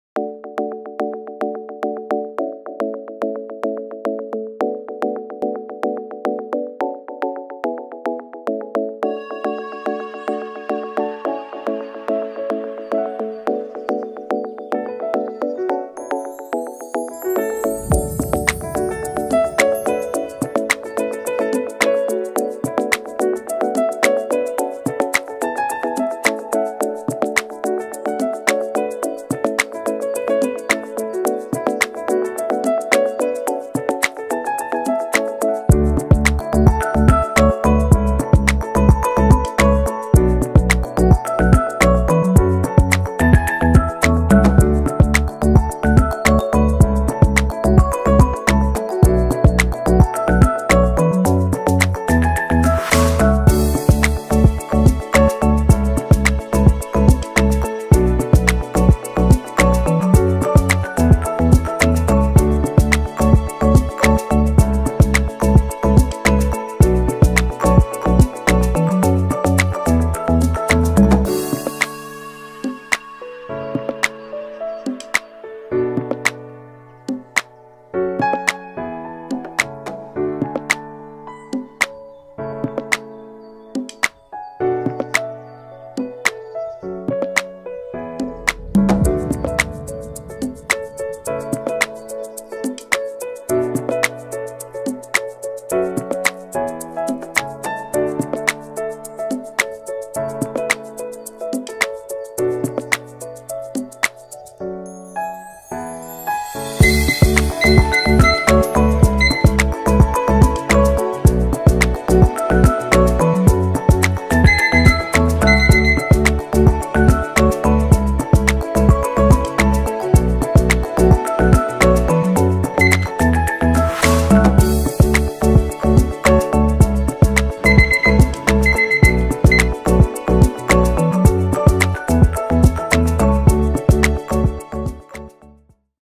Carbaryl Test - Detection of Carbaryl in Watermelon - BALLYA
The Carbaryl Test For Agricultural Products is based on the Colloidal Gold Immunochromatography to detect the Carbaryl residues in agricultural products. This kit can be applied for on-site or lab rapid testing by various departments.